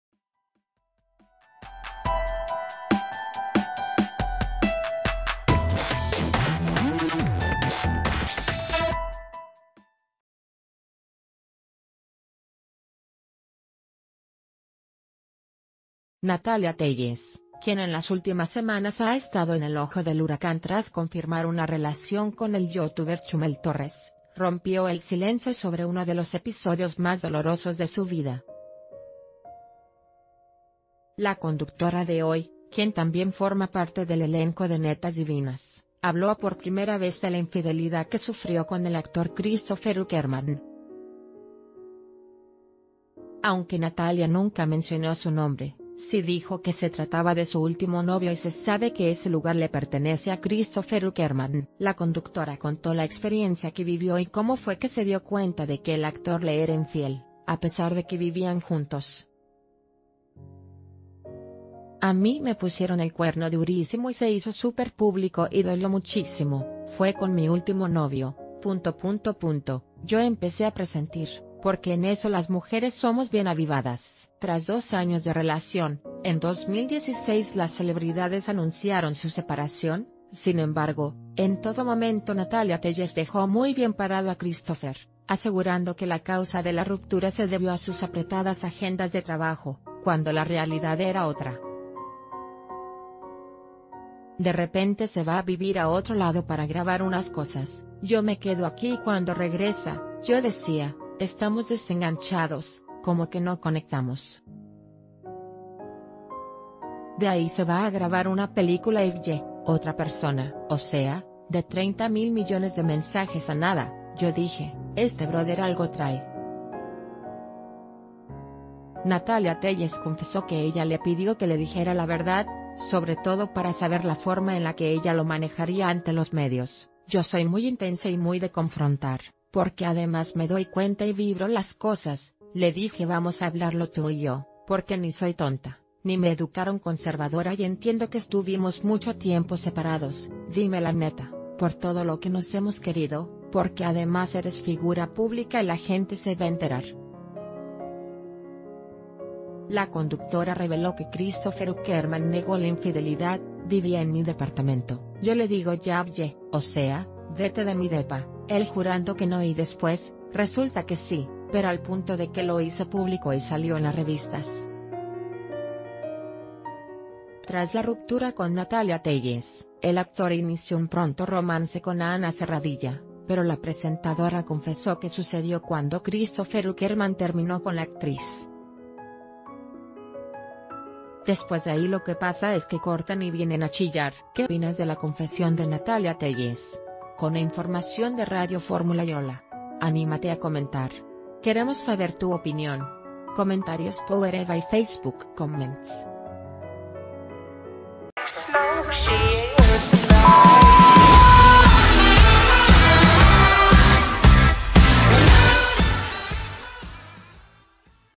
Natalia Téllez abre su corazón y confiesa infidelidad (FOTO)
Natalia Téllez, quien en las últimas semanas ha estado en el ojo del huracán tras confirmar una relación con el YouTuber Chumel Torres, rompió el silencio sobre uno de los episodios más dolorosos de su vida. La conductora de Hoy, quien también forma parte del elenco de Netas Divinas, habló por prime...